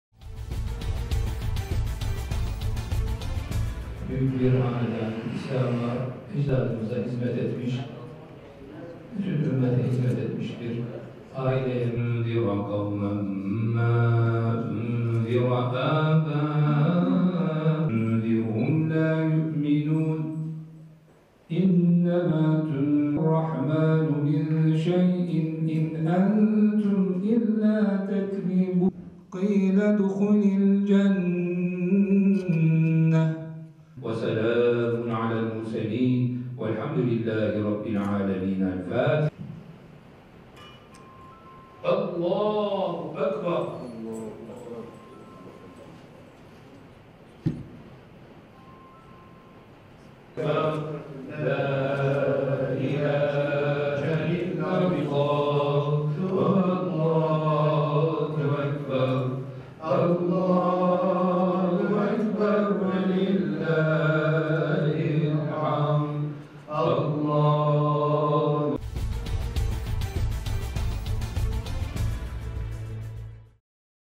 مصر العربية | تشييع جثمان حفيد السلطان العثماني "عبد المجيد" في نيويورك
جرت في مدينة نيويورك الأمريكية، مراسم تشييع جثمان "عثمان بيازيد عثمان أوغلو"، الرئيس الـ 44 لسلالة "آل عثمان"، الذي وافته المنية الجمعة الماضية، عن عمر يناهز 93 عامًا. وحضر المراسم، أمس الاثنين، "زينب عثمان" زوجة "عثمان أرطغرل أفندي" رئيس العائلة الـ 43، الذي سبق أن توفي في إسطنبول بتركيا عام 2009، وقنصل تركيا في نيويورك، أرتان يلجن، إلى جانب بعض أعضاء السلالة العثمانية، ولفيف من الجالية التركية. وقالت "زينب عثمان" للأناضول، إن عثمان أوغلو، وهو حفيد السلطان عبد المجيد الأول (السلطان الـ31)، "كان رجلاً متواضعًا، معربةً عن حزنها العميق لوفاته".